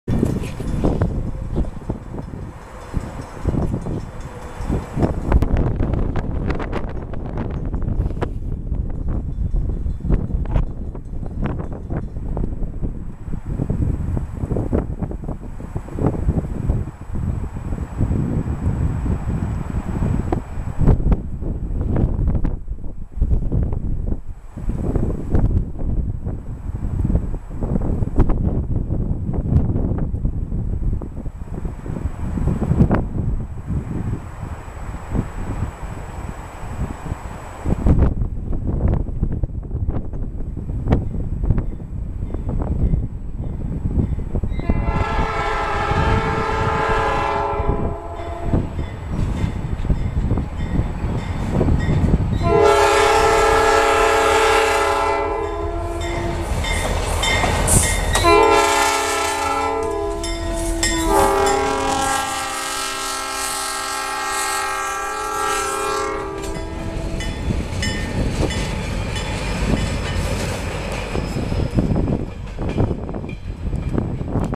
SLR in Falmouth
St Lawrence & Atlantic local 512 led by GP-40 3007 is returning from East Deering after dropping 2 loads at B&M Beans and lifting one empty.